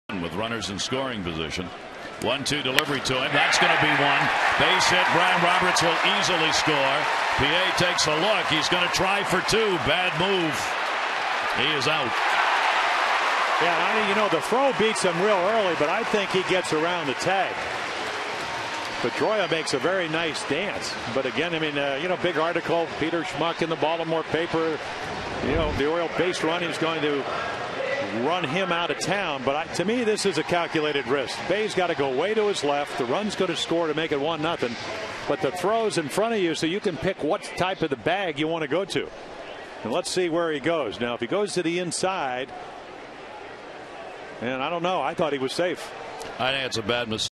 2009/07/01 Pie's RBI single
BOS@BAL: Pie singles to left to score Roberts